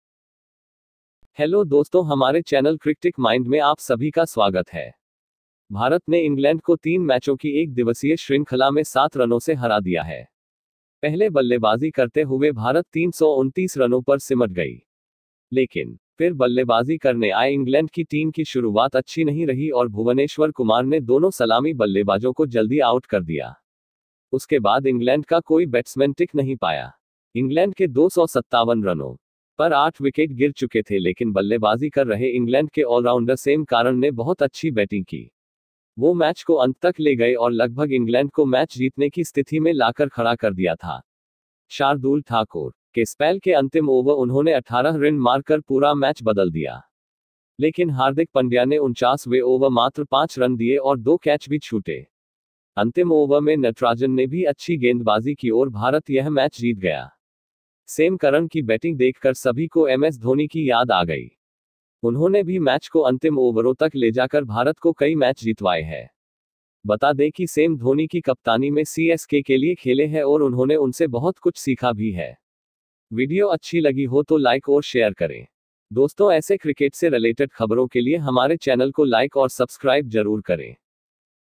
Ind vs England 3rd ODI Highlights | Sam Curran played the brilliant knock.
India beats England with 7 runs in a thrilling encounter. Sam Curran played a brilliant knock. Shardul Thakur and Bhuvneshwar Kumar bowled well in their 10 over spell.